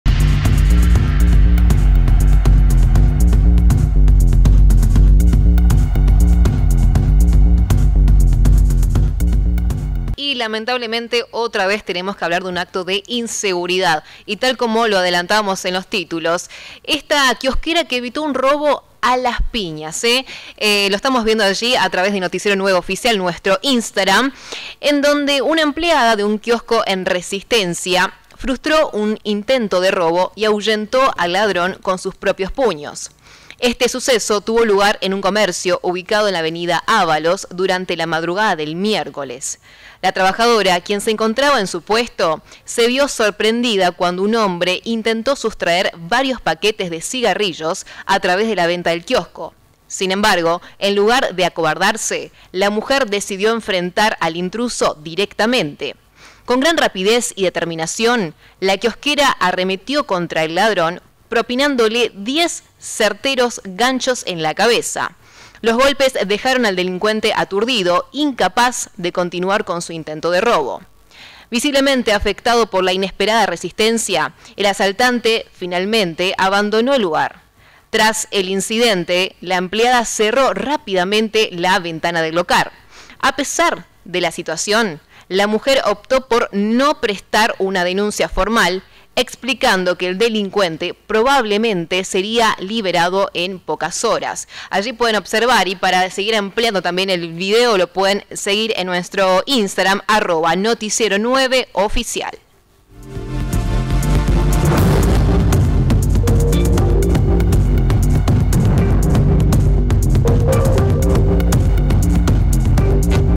KIOSQUERA “A LAS PIÑAS” EVITÓ UN ROBO - NOTICIERO 9
Una empleada de un kiosco en Resistencia frustró un intento de robo y ahuyentó al ladrón con sus propios puños. Este suceso tuvo lugar en un comercio ubicado en la avenida Ávalos durante la madrugada del miércoles.

La trabajadora, quien se encontraba en su puesto, se vio sorprendida cuando un hombre intentó sustraer varios paquetes de cigarrillos a través de la ventana del kiosco. Sin embargo, en lugar de acobardarse, la mujer decidió enfrentar al intruso directamente.

Con gran rapidez y determinación, la kiosquera arremetió contra el ladrón, propinándole diez certeros ganchos en la cabeza. Los golpes dejaron al delincuente aturdido, incapaz de continuar con su intento de robo.

Visiblemente afectado por la inesperada resistencia, el asaltante finalmente abandonó el lugar. Tras el incidente, la empleada cerró rápidamente la ventana del local. A pesar de la situación, la mujer optó por no presentar una denuncia formal, explicando que el delincuente probablemente sería liberado en pocas horas.